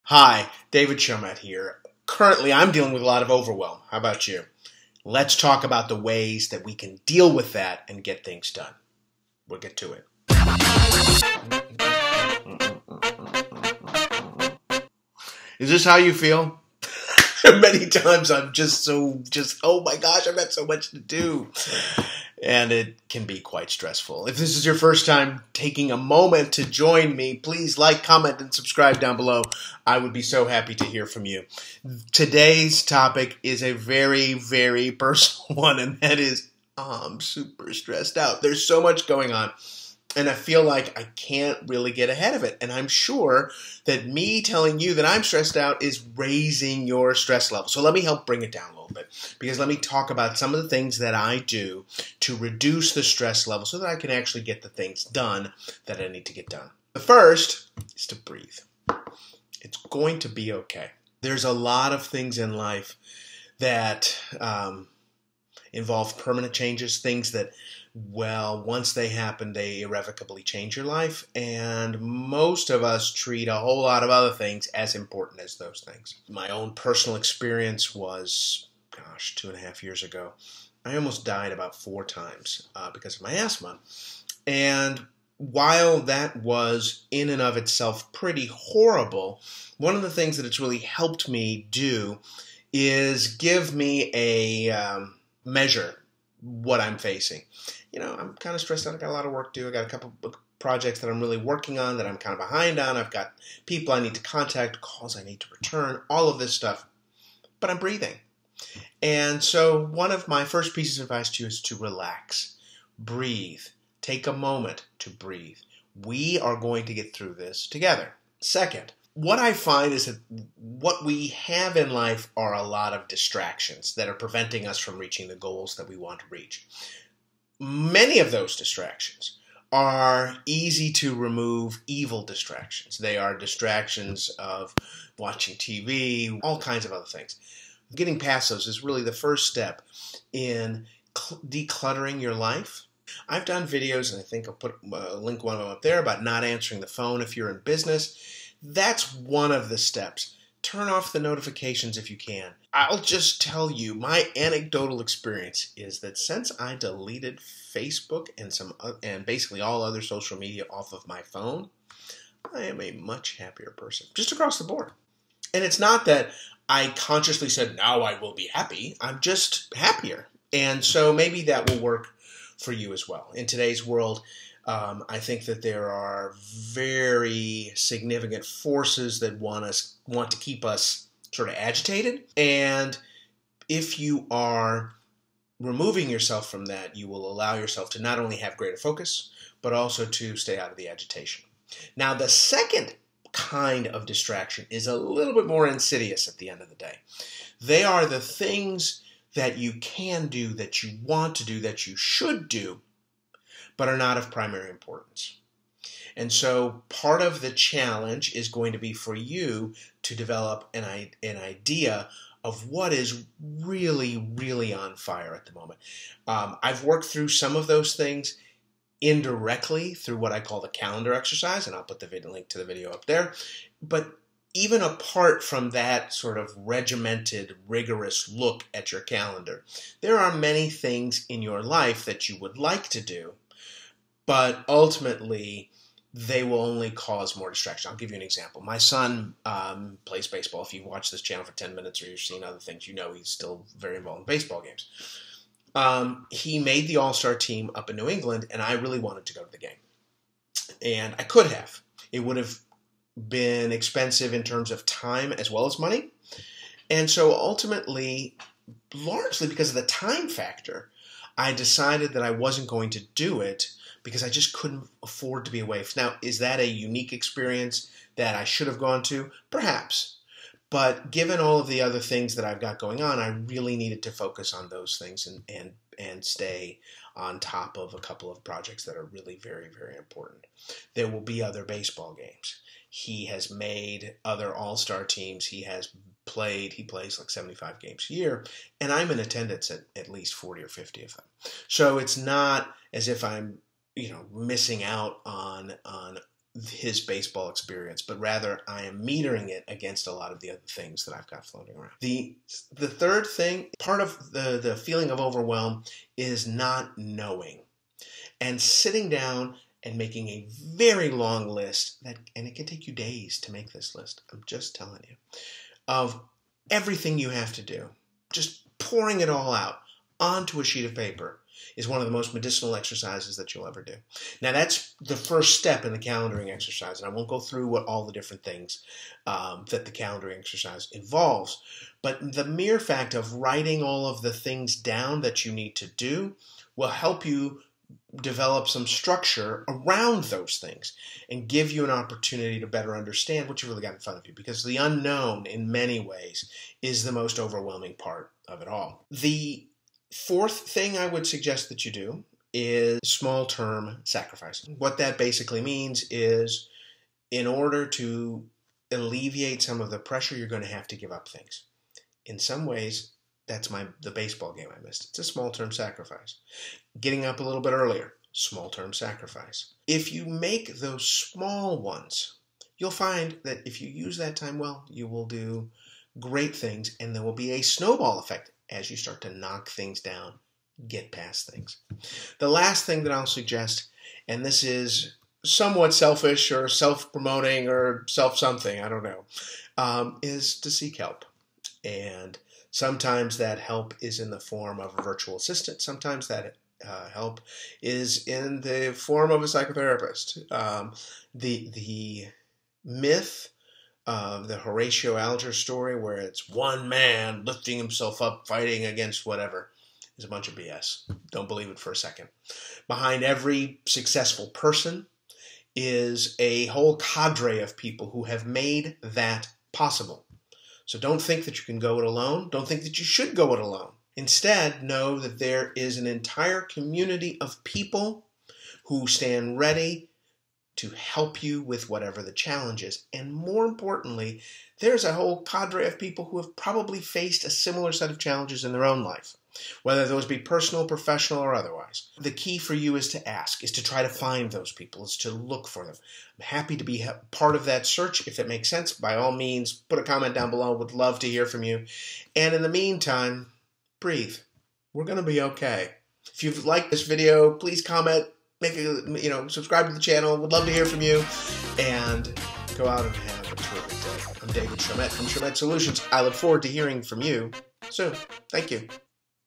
Managing Overwhelm in Today's Chaotic Times: Small Business Owners Mindset
It's hard managing overwhelm in today's business climate. This video contains advice for the  small business owner dealing with a super full plate. Managing overwhelm is not easy, but it is possible with a little focus and attention.